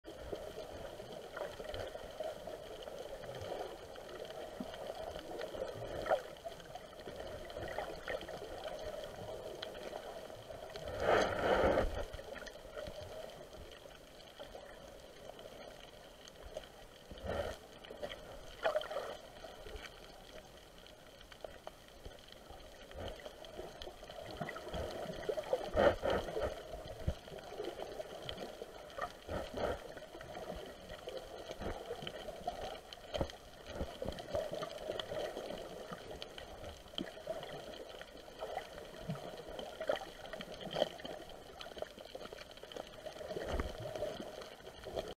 Rainbow Parrotfish Swimming in the Florida Keys
A rainbow parrotfish (Scarus guacamaia) swimming over the seafloor at Alligator Reef near Islamorada, Florida in the Florida Keys.